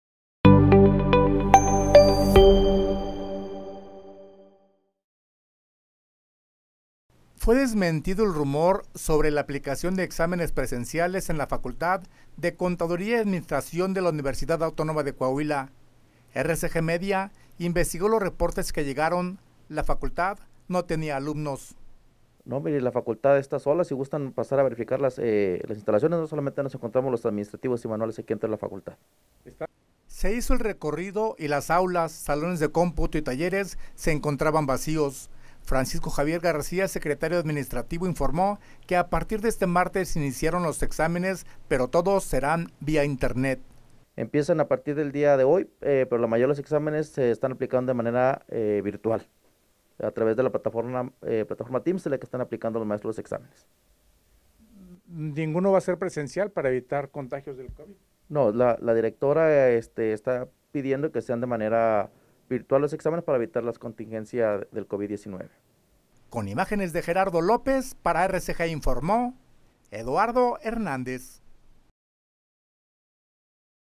FCA no realiza exámenes presenciales
🔴  La FCA no realiza exámenes presenciales como se rumoró, se recorrieron las instalaciones y no hubo presencia de alumnos